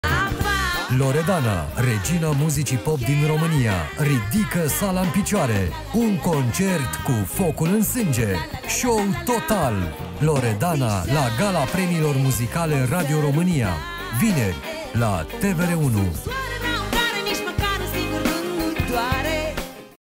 Concert Loredana (Premiile Muzicale Radio România)
Anul acesta, Loredana a primit trofeul "Artistul anului 2013" la Premiile Muzicale Radio România. Pe 2 Mai, TVR1 vă oferă concertul pe care l-a susţinut aceasta imediat după eveniment.